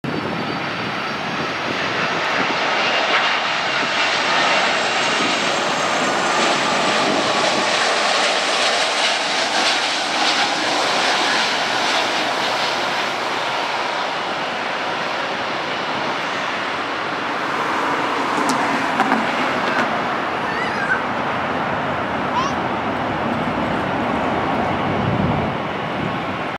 China Eastern Airlines - Airbus A330-200 Landing at Los Angeles (LAX)
Aircraft Built: 1998
Airliner Found: 1988

Note: China's First New Livery with Rare A330 to USA.